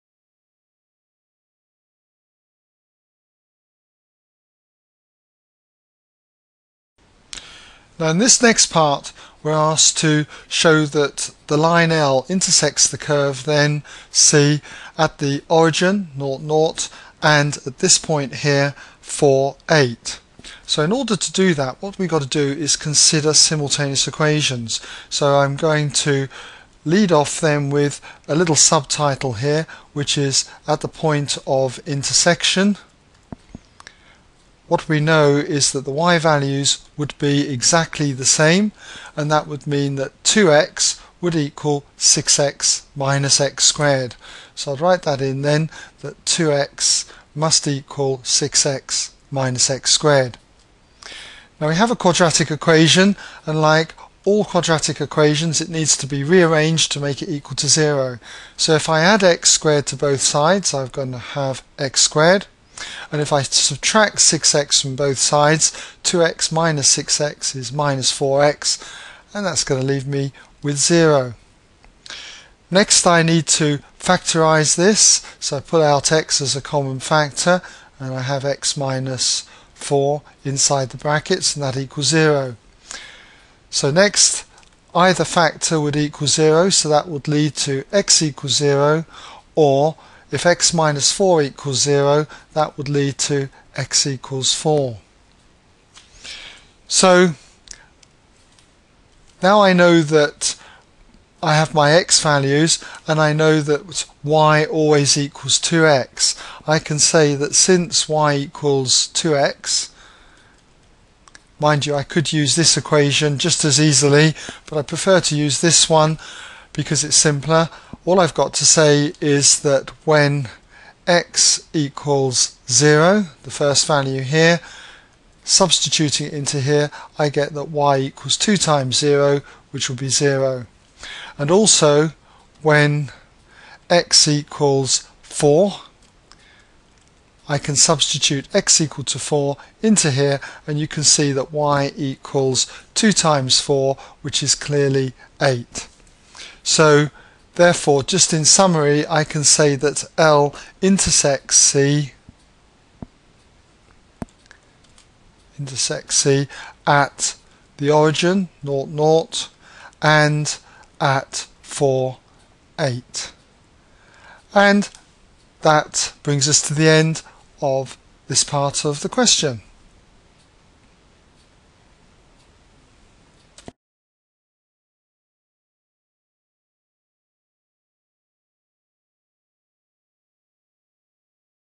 A-Level Maths Edexcel C2 January 2008 Q7b ExamSolutions
This question is on the intersection of a line and a curve. To see this and other questions goto ExamSolutions